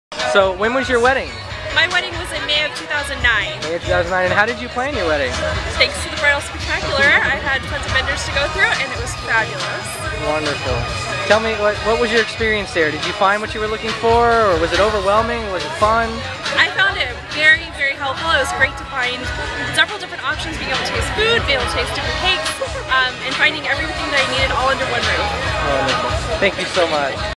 Bride Talks About How Bridal Spectacular Helped Plan Her Las Vegas Wedding
Interview with Las Vegas bride at a Bridal open house where she talks about how Bridal Spectacular helped her plan her wedding.